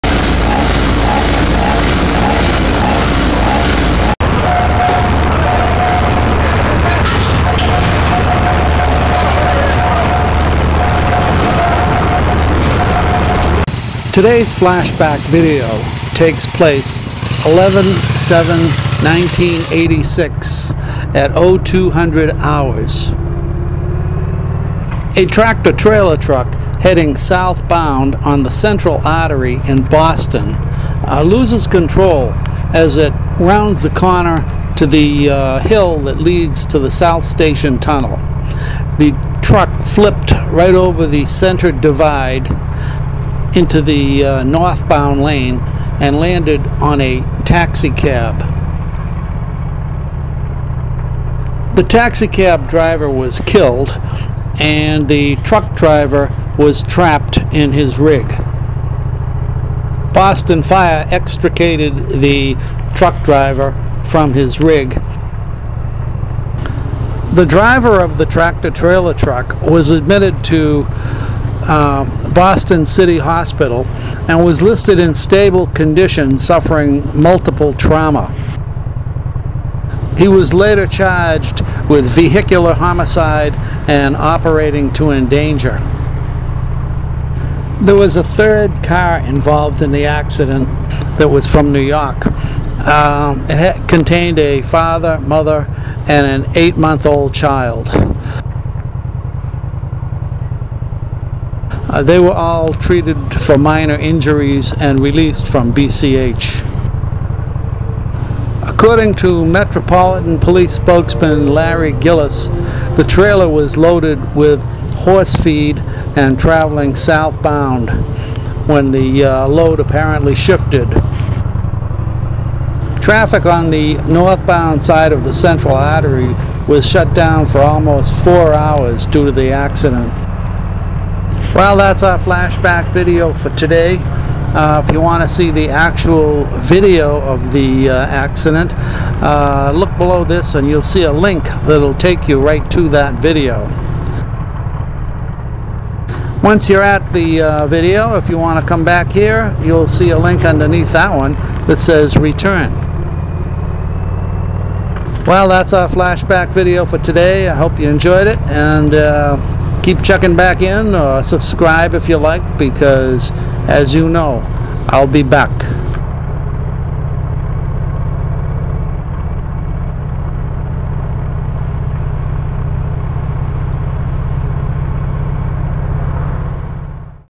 Central Art. Boston TT MVA flashback
Central Art. Boston TT MVA flashback  11/07/1986  02:00 Hrs.  TT vs taxi and car / fatal